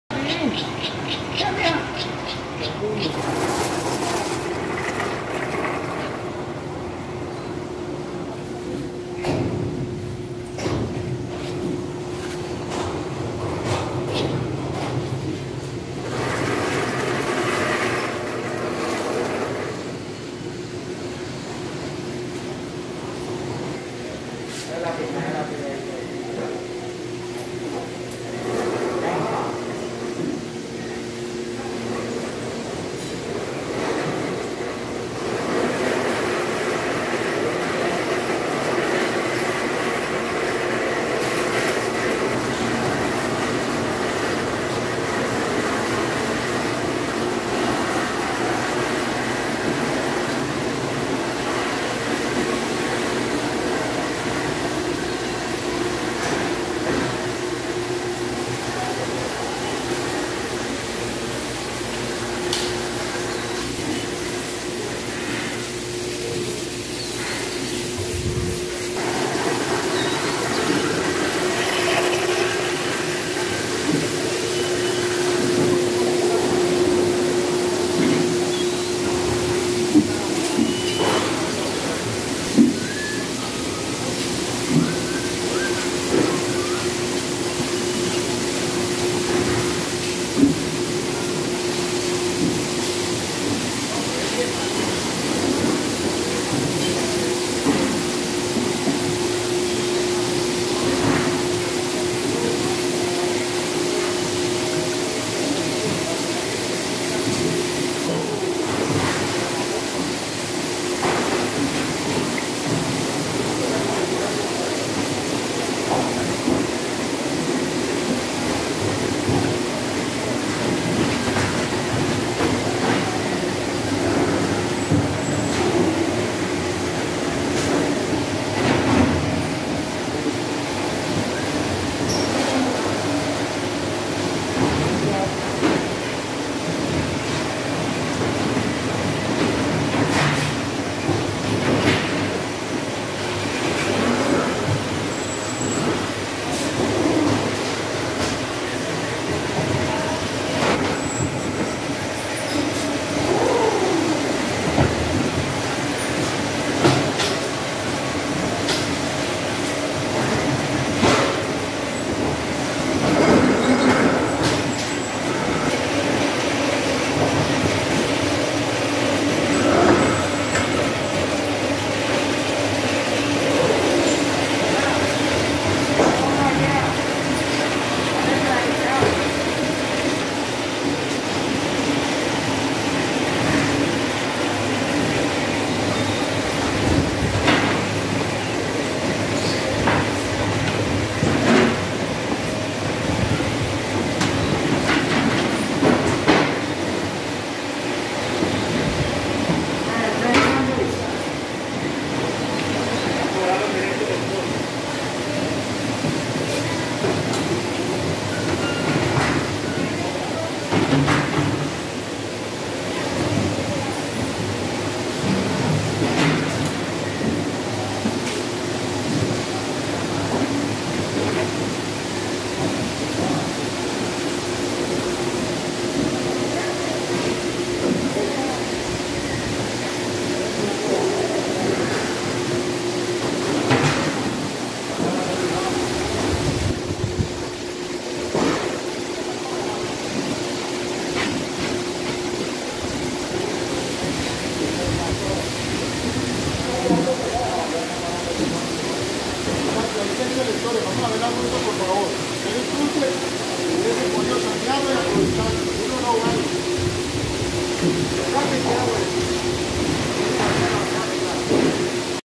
PROCESO FRUTA DE EXPORTACIÓN
En esta planta se realiza el proceso de preparación, clasificación y embalaje de la fruta para exportación a mercados internacionales, con los mejores estándares de calidad y normas de certificación para llevar al mundo un excelen producto alimenticio.